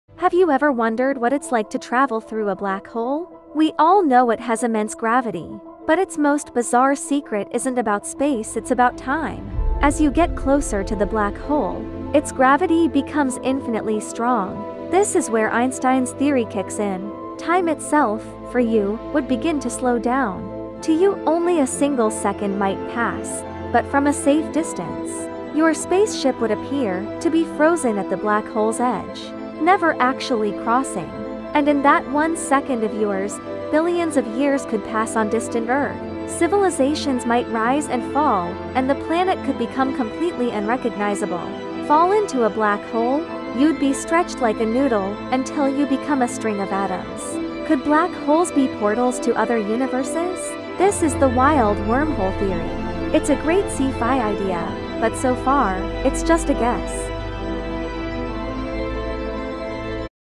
A Minute Through a Black Hole: Does Time Really Bend?
Ever wondered what it's like inside a black hole? If humans could travel through one, what would happen to time? Get ready to explore the weirdest object in the universe in just 60 seconds!
​Video Breakdown:
​Why do black holes warp time?
​What happens if you fall into one?
​Could they be secret doorways to other universes?